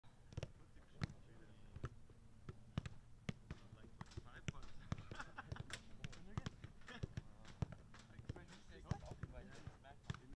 Harry Kane gets to know Thomas Müller 😃
Harry Kane and Thomas Müller share their first ball contacts during training - and soon on the pitch!